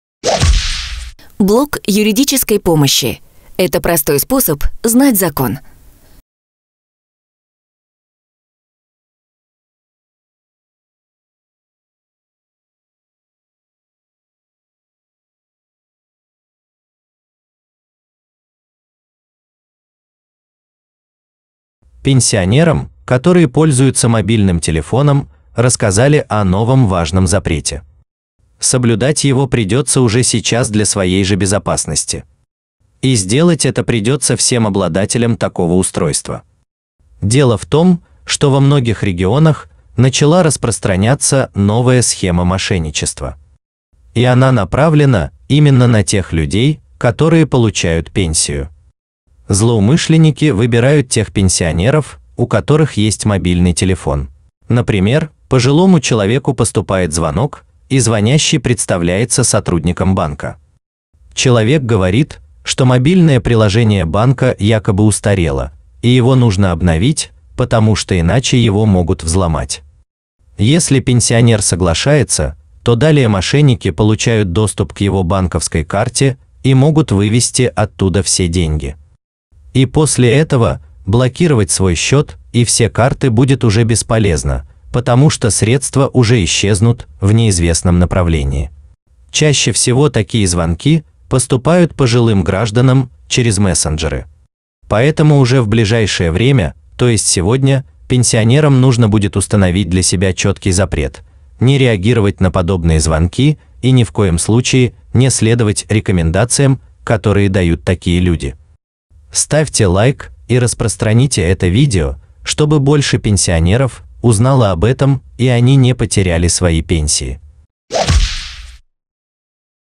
ПОЛНЫЙ ЗАПРЕТ Пенсионерам для Сохранения Пенсий!
В этом выпуске:
Пенсионерам, которые пользуются мобильным телефоном, рассказали о новом важном запрете. Соблюдать его придется уже сейчас  для своей же безопасности. И сделать это придется всем обладателям такого устройства. Дело в том, что во многих регионах начала распространяться новая схема мошенничества. И она направлена именно на тех людей, которые получают пенсию. Злоумышленники выбирают тех пенсионеров, у которых есть мобильный телефон.
Например, пожилому человеку поступает звонок, и звонящий представляется сотрудником банка. Человек говорит, что мобильное приложение банка якобы устарело, и его нужно обновить, потому что иначе его могут взломать. Если пенсионер соглашается, то далее мошенники получают доступ к его банковской карте и могут вывести оттуда все деньги. И после этого блокировать свой счет и все карты будет уже бесполезно, потому что средства уже исчезнут в неизвестном направлении.
Чаще всего такие звонки поступают пожилым гражданам через мессенджеры.  Поэтому уже в ближайшее время, то есть сегодня, пенсионерам нужно будет установить для себя четкий запрет: не реагировать на подобные звонки и ни в коем случае не следовать рекомендациям, которые дают такие люди.